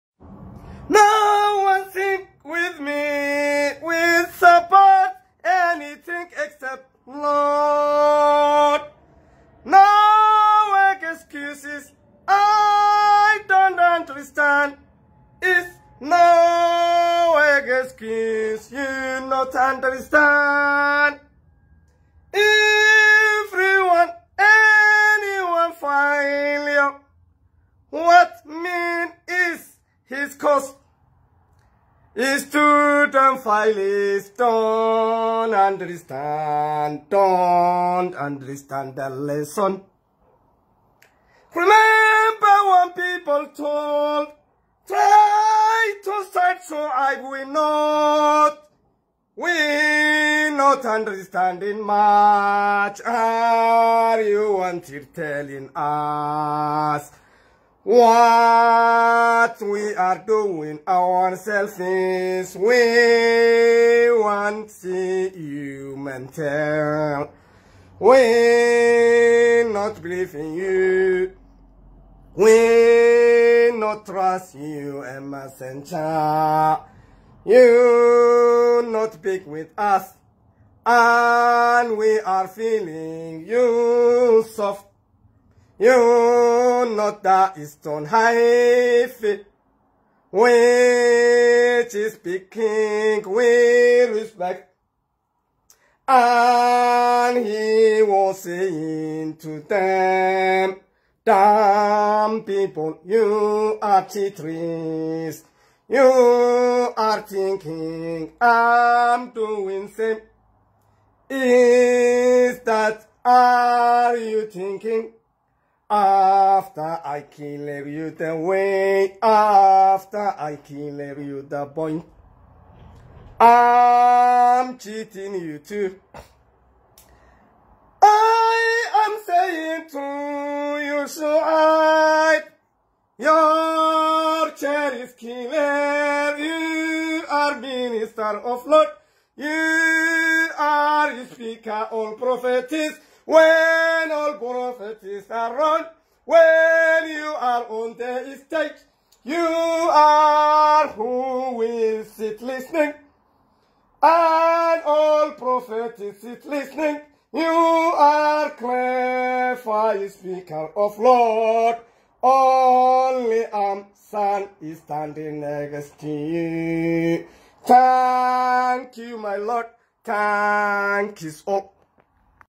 No excuses I don't understand !
No excuses I understand
Is no excuse you not understand
Everyone anyone failure
What mean is his cost
Student fails don't understand
Don't understand the lesson

Remember when people told
Try to said shuib we not
We not understanding much
Are you wanted telling us
What we are doing our selfs
We want see you mental
We not believing you
We not truth you a messenger
You not big with us
And we are feeling you soft
You not that stone heavy
Which is big king we respect
And he was saying to them
Damp people you are cheaters
Are you thinking am doing same
Is that are you thinking
After I clear you the way
After I clear the point
Am cheating you too

I am saying to you shuib
Your chair is clear
You are minter of lord
You are speaker all prophets
When all prophets around
When you are on the stage
You are who we sit listening
And all prophets sit listening
You are cleaver speaker of lord
Only am son standing next to you